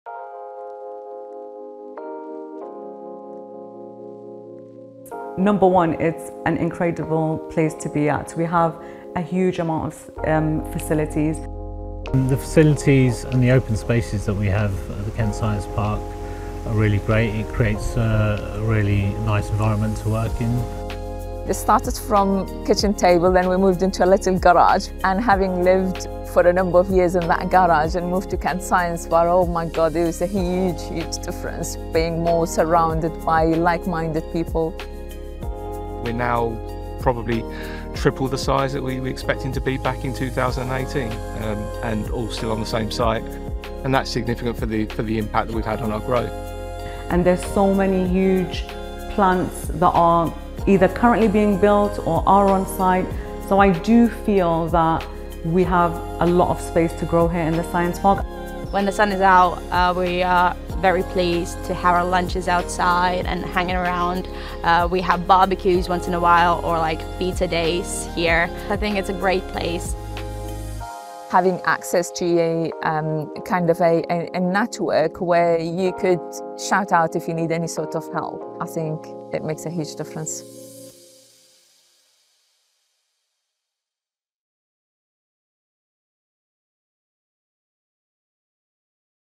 Kent Science Park | Part of Pioneer Group – Office & Lab Space, Business Support, Events
Discover Kent Science Park – part of the Pioneer Group ecosystem – where science, technology and business come together to grow.

Based in Sittingbourne, Kent Science Park offers state-of-the-art office and laboratory space with access to expert-led business support, on-site events, and a thriving life sciences and innovation community.

What you’ll find at Kent Science Park:
• Fully serviced office and lab space
• Business support programmes: Launch, Scale, and Investment Showcase
• Regular networking and innovation events
• Access to funding, mentoring, and facilities through Pioneer Group